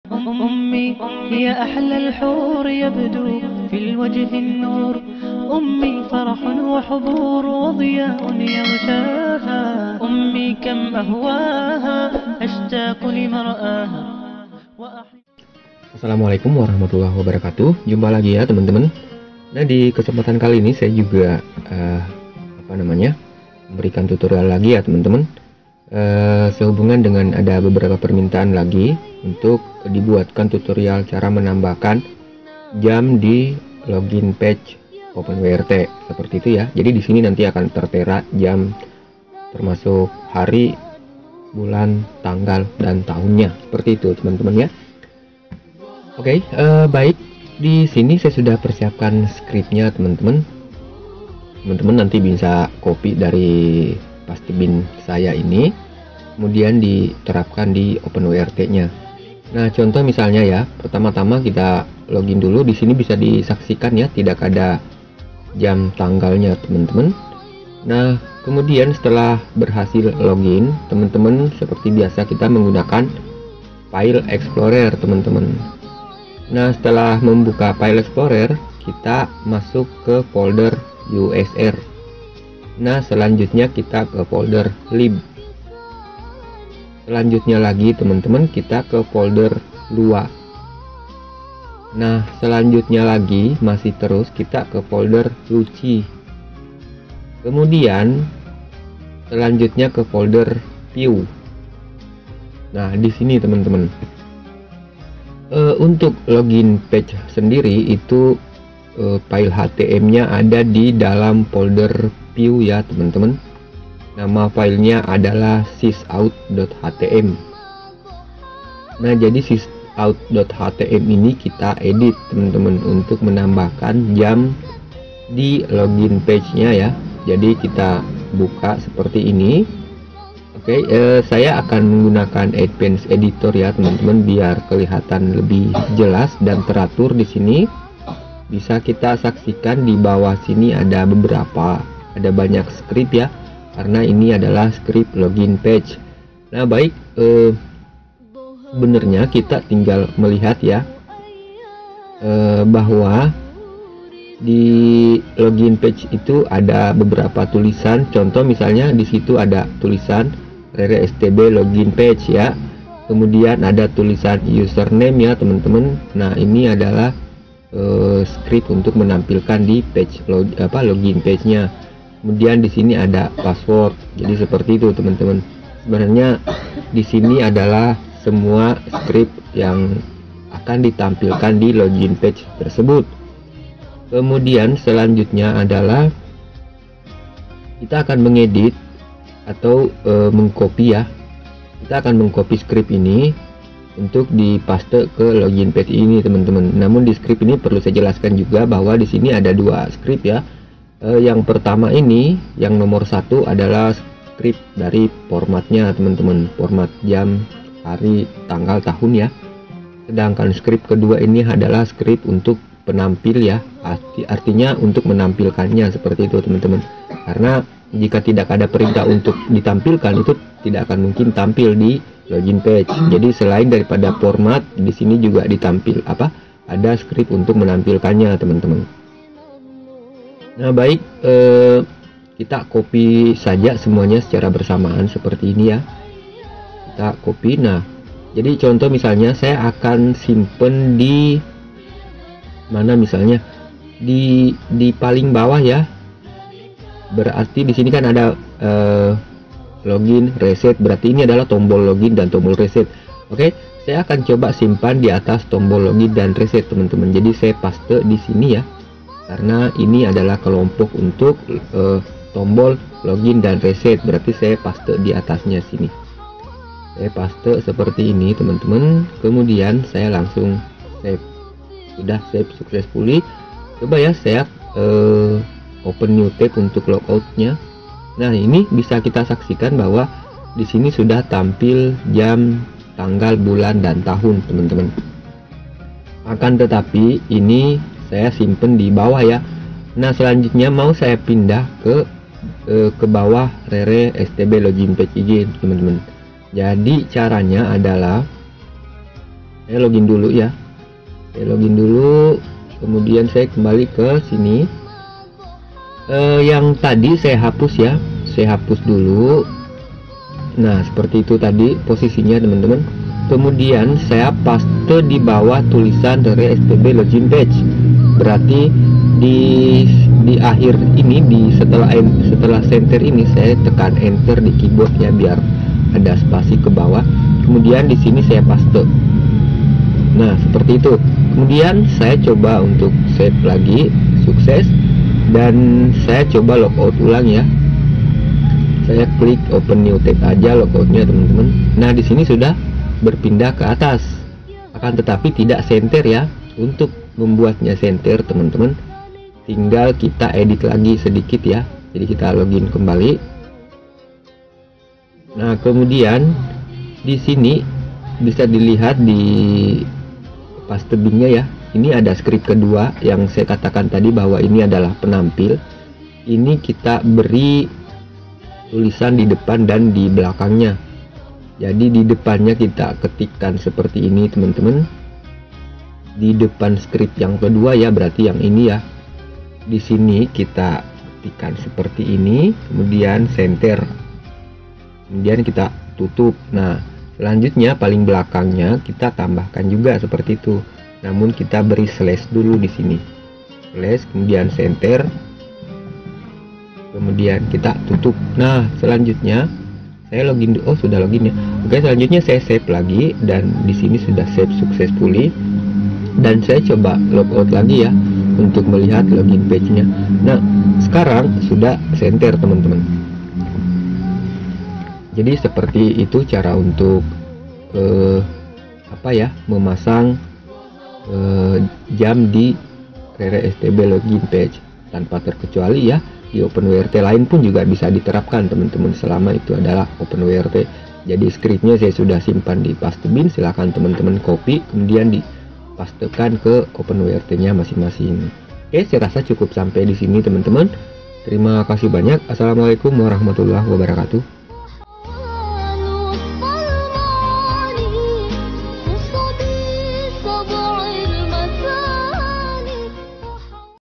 Menampilkan Jam, Hari, Tanggal, Bulan dan Tahun di Login Page OpenWrt | REYRE-STB
Tutorial sederhana dan script sederhana untuk menampilkan waktu di login page openwrt